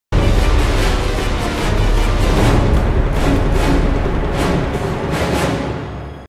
AP_Ending Aurora Pictures
Pada hujung drama TV nanti. InsyaAllah akan keluar benda ni..Sangat mengujakan mempelajari sony vegas pro 11 :)